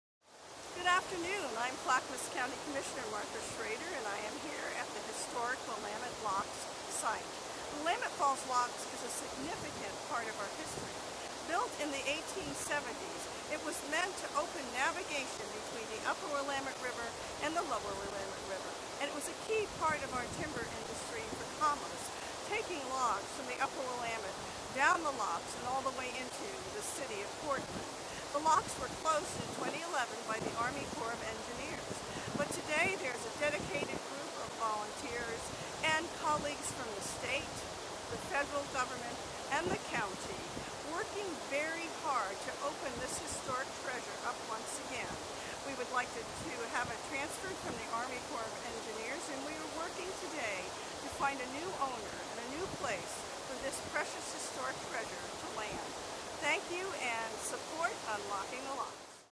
360 Video: Commissioner Martha Schrader discusses the Willamette Falls Locks
Get a 360 view of the Willamette Falls Locks (and from the air!) as ClackCo Commissioner Martha Schrader discusses the Locks’ history, and the county’s desire to see the Locks reopened.
This is part of the way that ClackCo chooses to Honor, Utilize, Promote and Invest in our natural resources ... one of our five key strategic priorities!